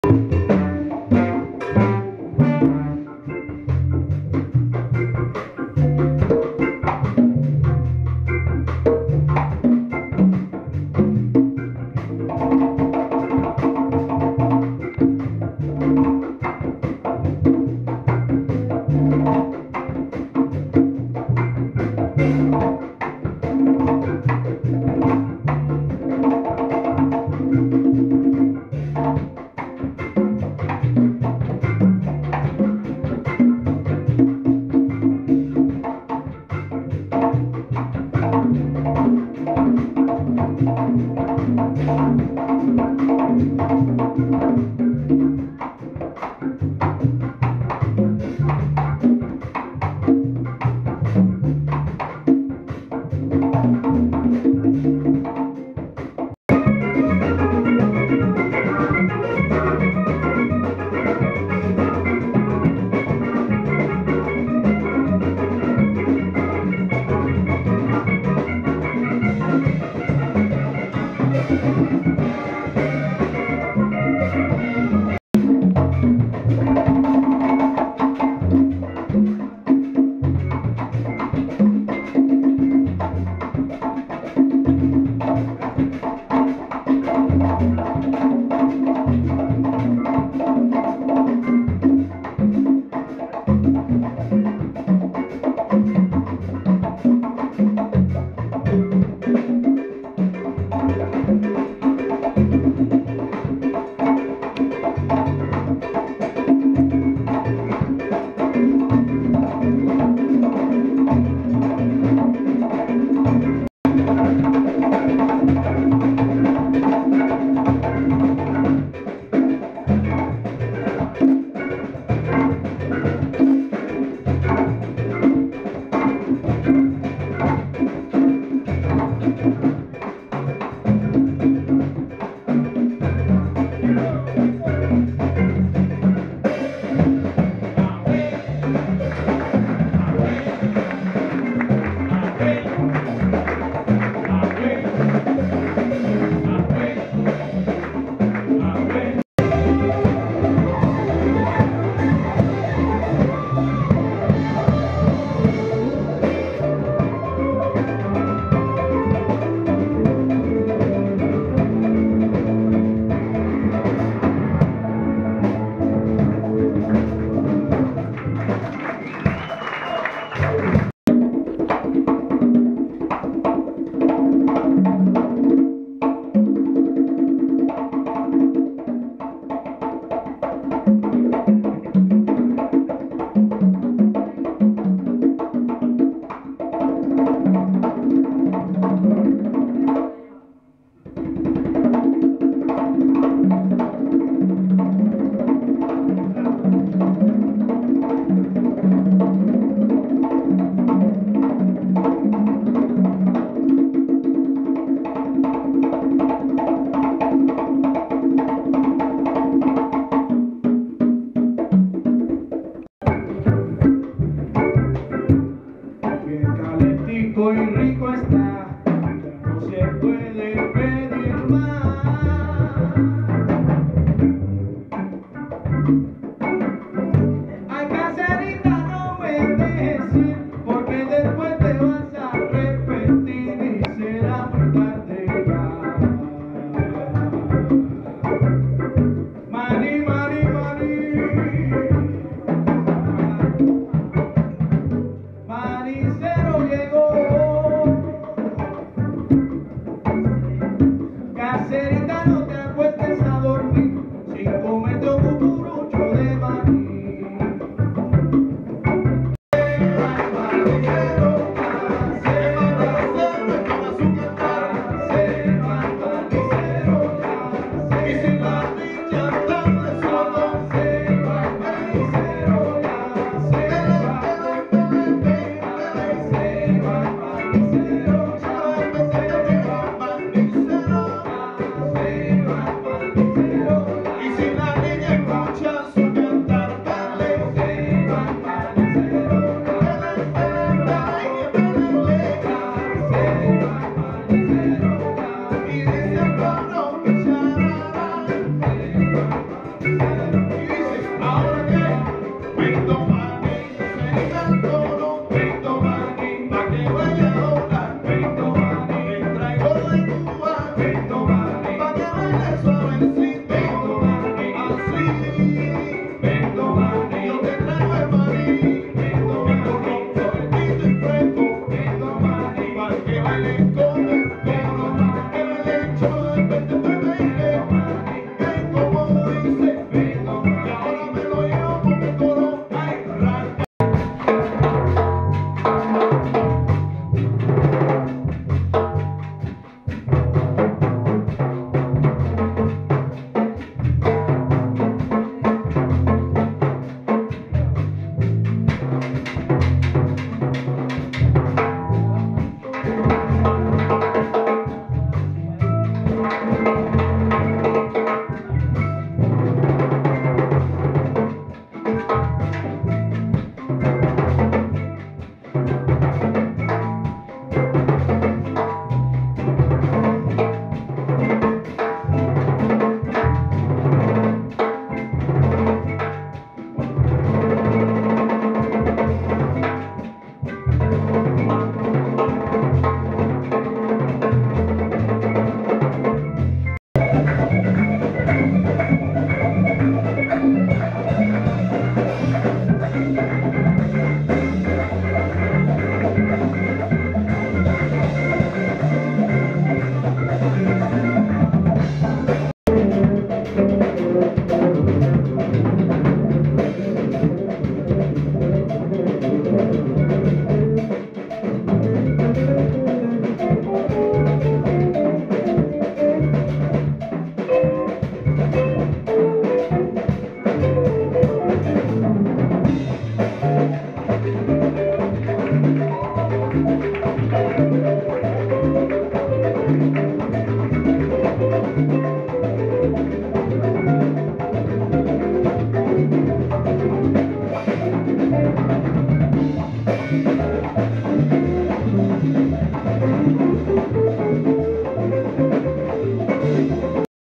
AFRO CUBA JAZZ AT GIANT STEPS SVENDBORG.DK
15.sep. 2011 at Giant Steps JAZZ CLUB, Svendborg.dk
African drummings rhythm complexity very high and intelligent sort of art.
This bands music is a mix of folk-, dance-, jazz- and fineartmusic.